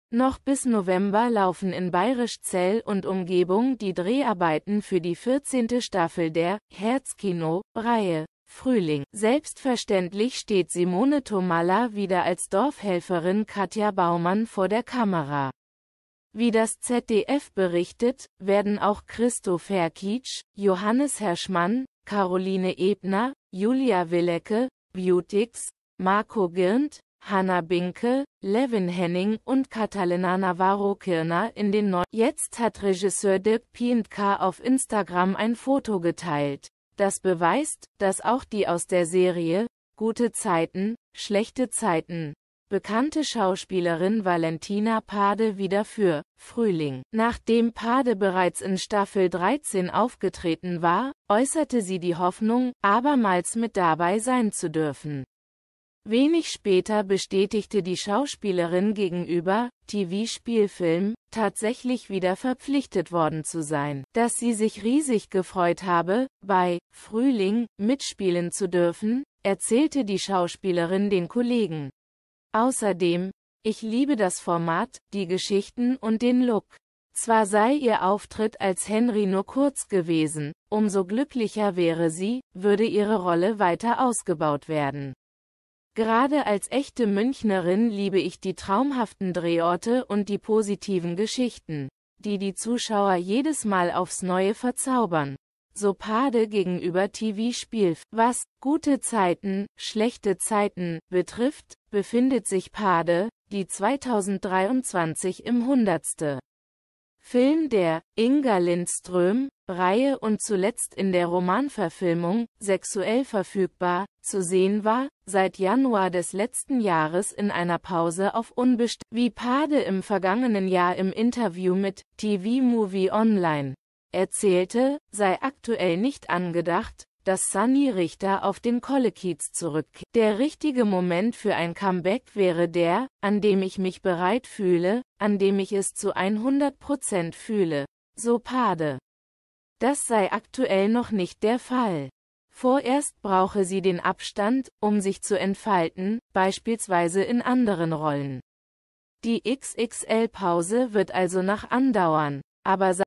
GZSZ-Star Valentina Pahde: Erstes Foto ihres Serien-Comebacks veröffentlicht
Noch bis November laufen in Bayrischzell und Umgebung die Dreharbeiten für die 14. Staffel der „Herzkino“-Reihe „Frühling“.

Selbstverständlich steht Simone Thomalla wieder als Dorfhelferin Katja Baumann vor der Kamera. Wie das ZDF berichtet, werden auch Kristo Ferkic, Johannes Herrschmann, Caroline Ebner, Julia Willecke (Beautx), Marco Girnth, Hanna Binke, Levin Henning und Catalina Navarro Kirner in den neuen Folgen zu sehen sein.